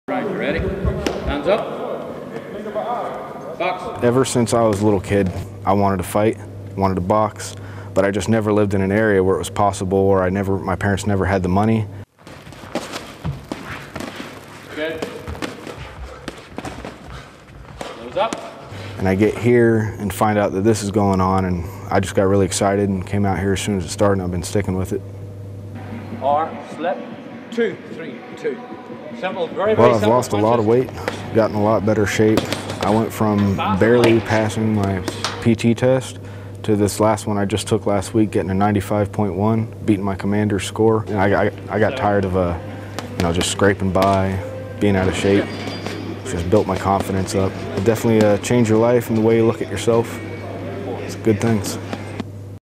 Float like a butterfly, sting like a bee
Want to get fit, but aren't sure how? Staff Sgt. Aaron Way shares how he got in shape by attending boxing classes.